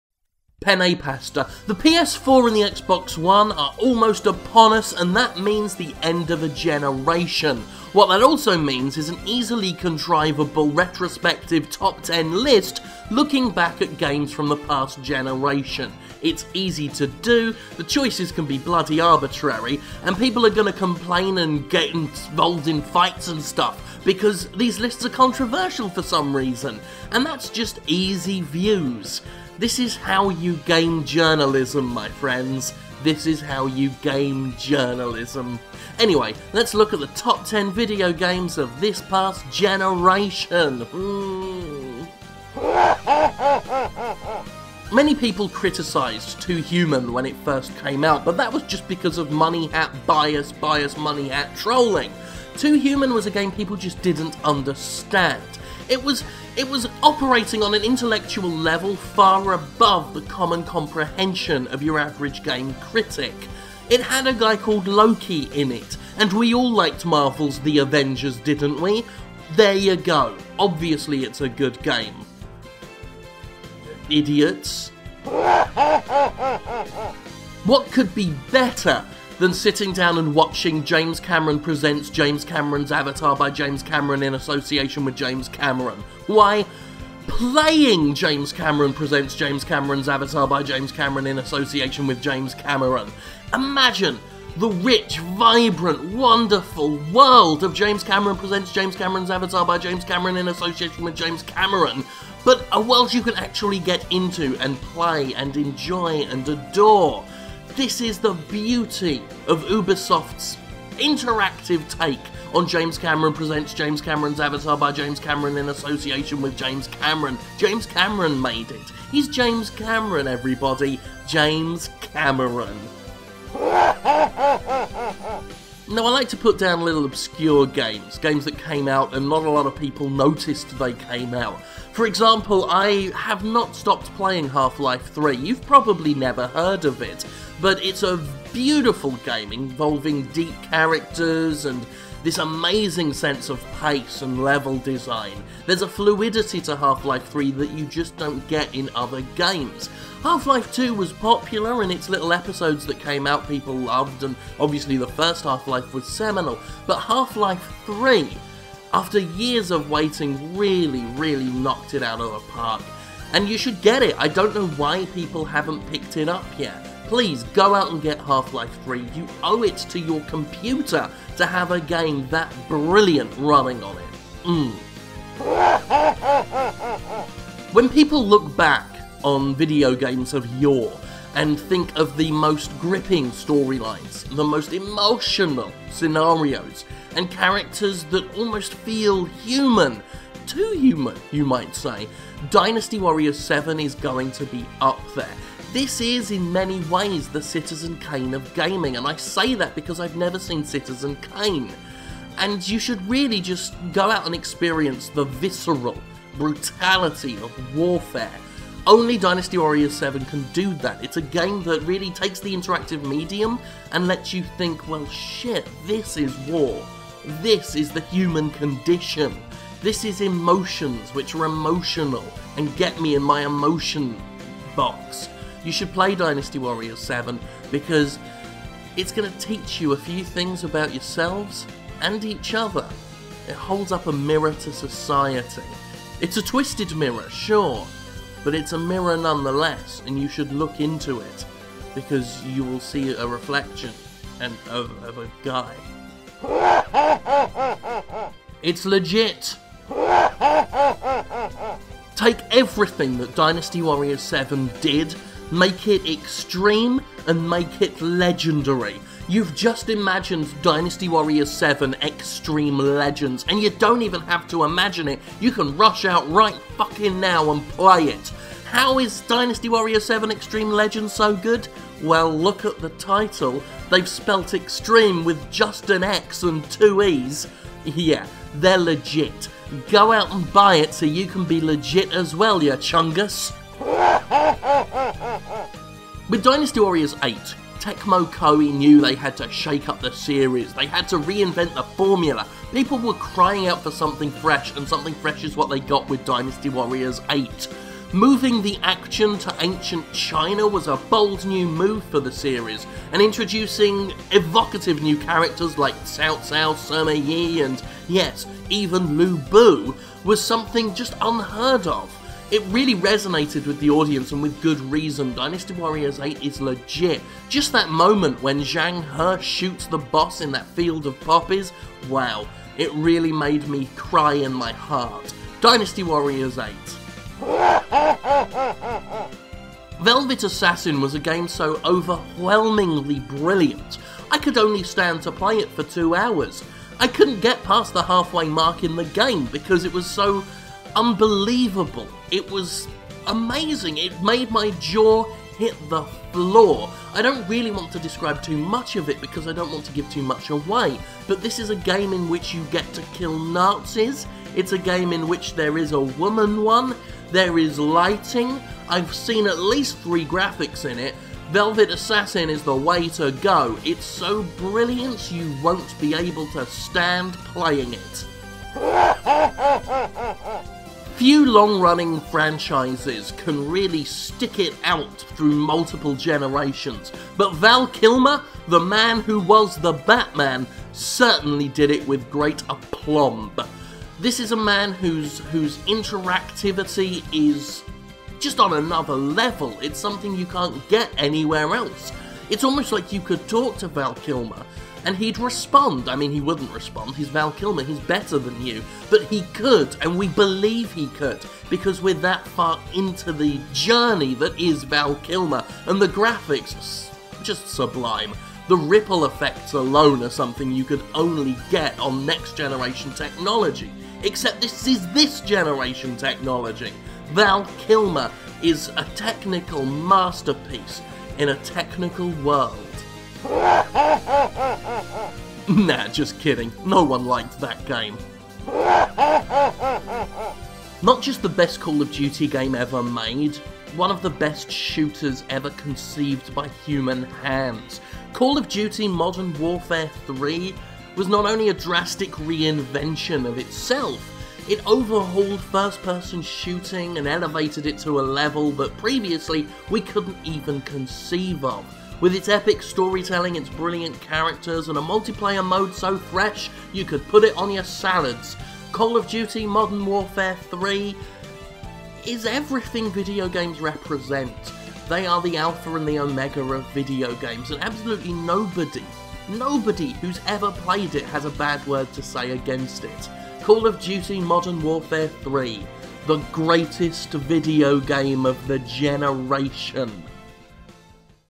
Top Ten Games of the GENERATION
With the PS4 and Xbox One on the horizon, let's look back and celebrate the past five years of quality gaming.